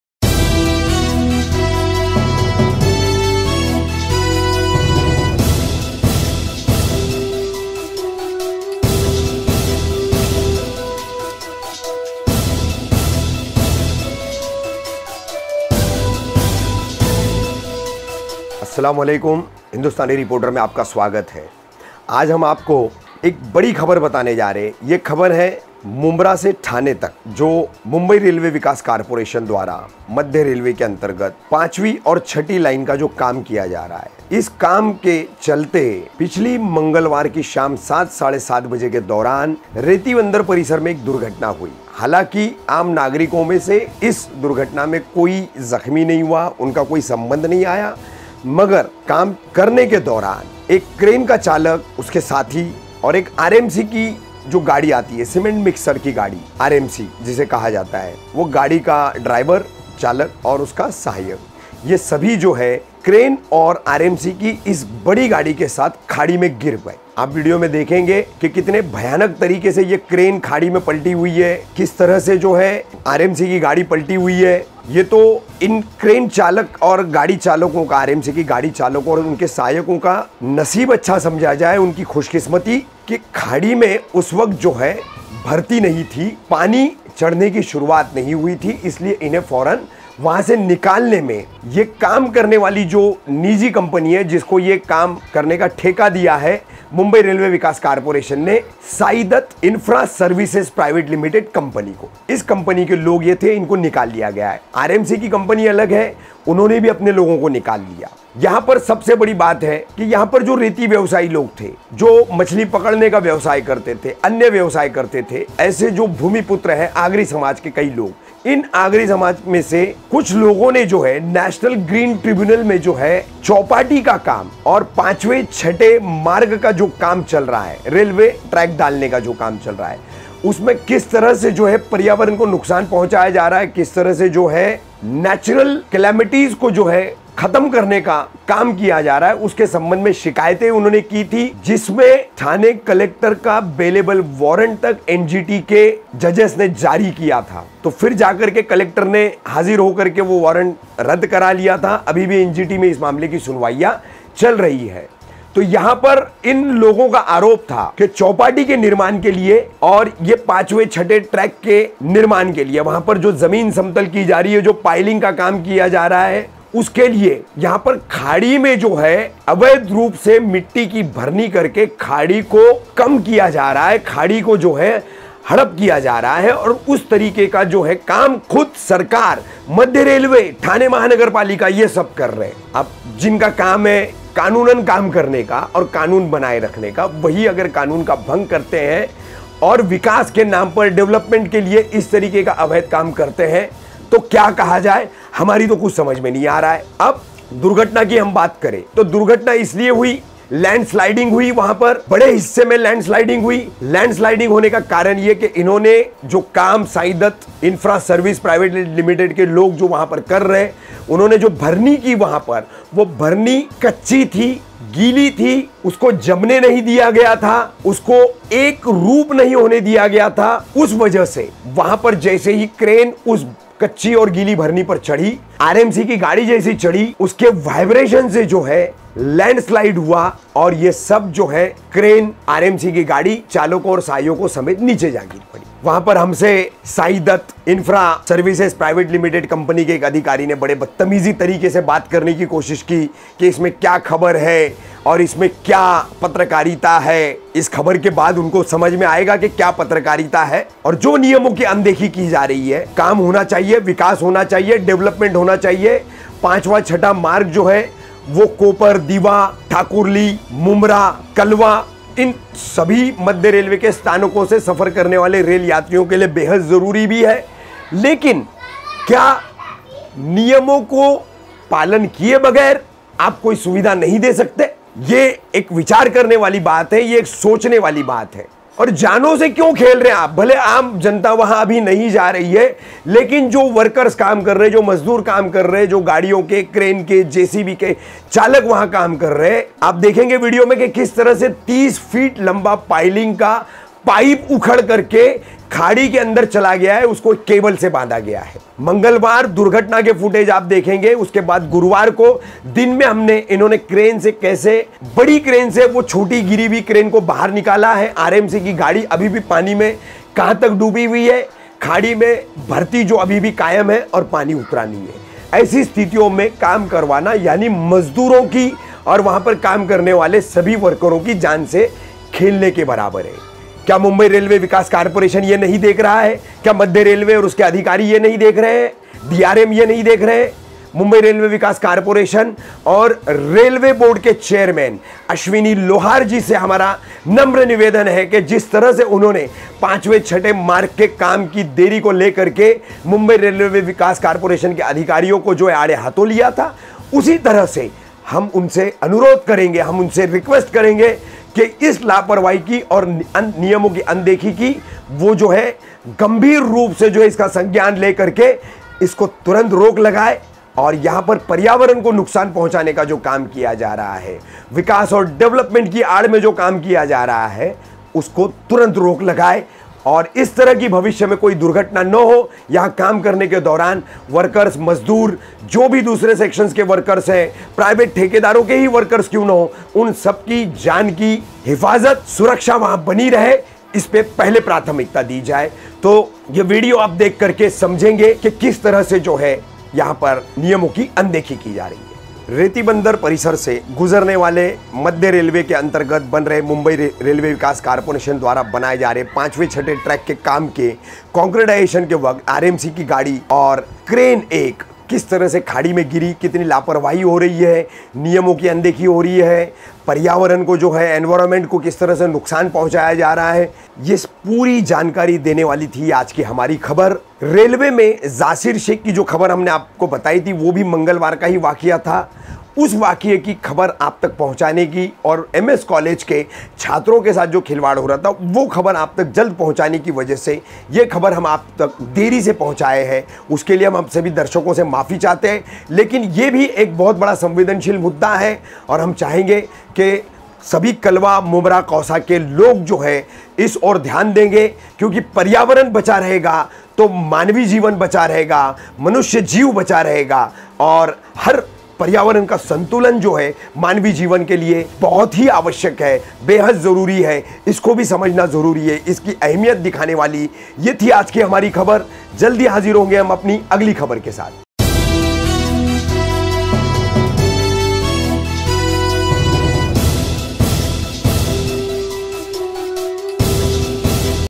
5th Aur 6th Railway Track Ke Kam Ke Dauran Retibundar Par Hua Bada Hadasa | Hindustani Reporter
5th Aur 6th Railway Track Ke Kam Ke Dauran Retibundar Par Hua Bada Hadasa

Crain Aur RMC Ki Gaadi Chalak Samet Landslide Ke Karan Gire Khadi Me

Environment Ke Niyamo Ko Taak Par Rakhkar Ki Jaa Rahi Hai Khadi Me Mitti - Debrij Ki Bharani

Contractor Saidatt Infra Service Barat Rahi Hai Kam Me Laaparwahi

MRVC Nahi De Rahi Hai Dhyan